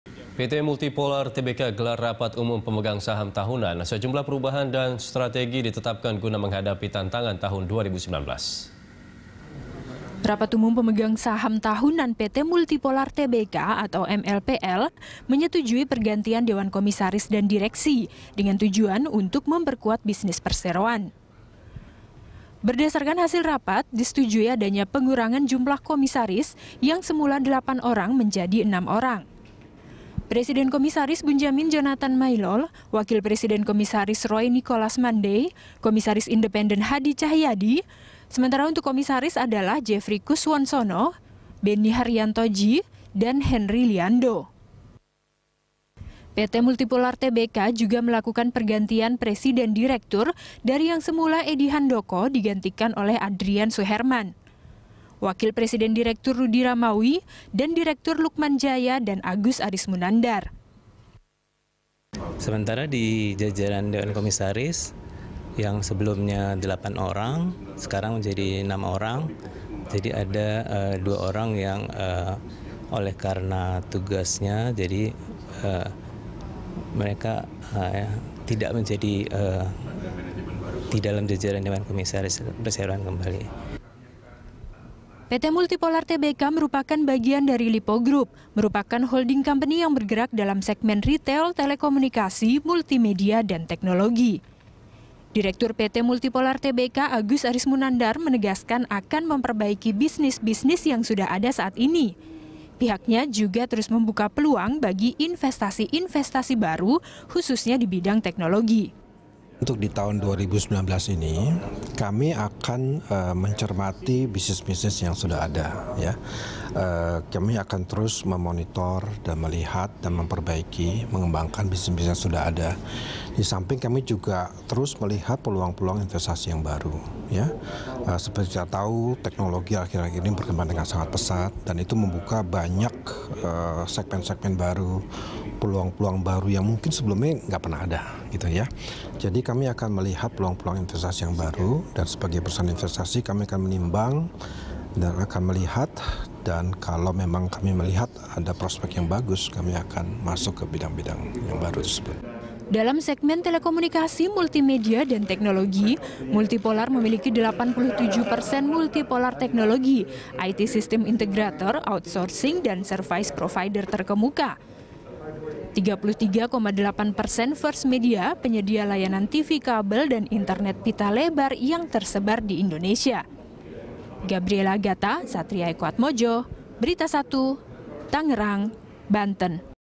RUPST Multipolar Setujui Pergantian Dewan Komisaris dan Direksi
PT Multipolar Tbk menggelar rapat umum pemegang saham tahunan. Sejumlah perubahan dan strategi ditetapkan guna menghadapi tantangan tahun 2019.

Facebook.com/BeritaSatu
Youtube.com/BeritaSatu